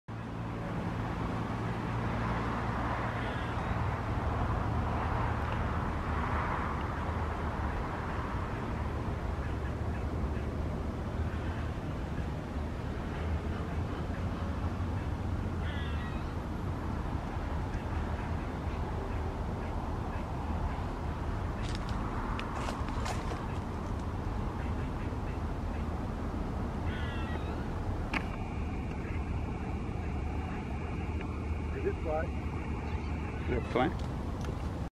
Summer Tanager at the Golden Gate Park
Don't we love walking by the Lily Pond and checking out every possible tree branch for that well-deserved surprise? Ooohhhh yeah...